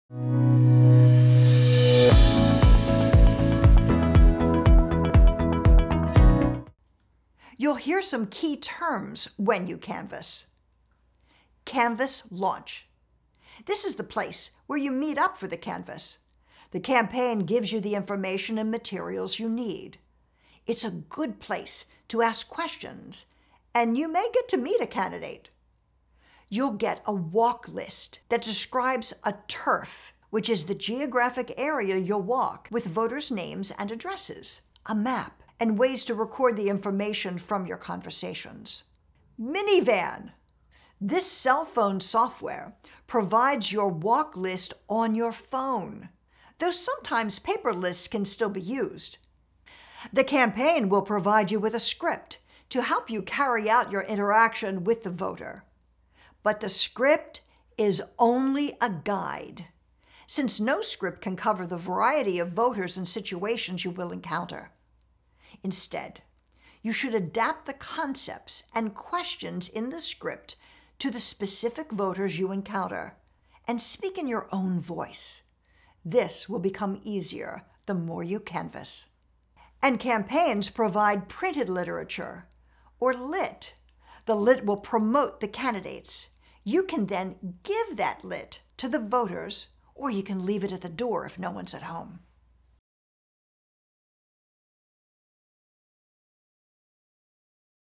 Canvassing Conversations: Part 04 - Key Terms
Canvassing Conversations is a tutorial to help give you all the basics you will need to begin canvassing for a candidate or issue. To go along with the video, you can access additional, helpful information in our written supplement

Narrated by 30 year, veteran, Philadelphia, TV news anchor Pat Ciarrocchi and written by experienced canvassers, this can be a valuable tool for campaigns to help train their volunteers.

Canvassing Conversations is an initiative of 31st Street Swing Left and Turn PA Blue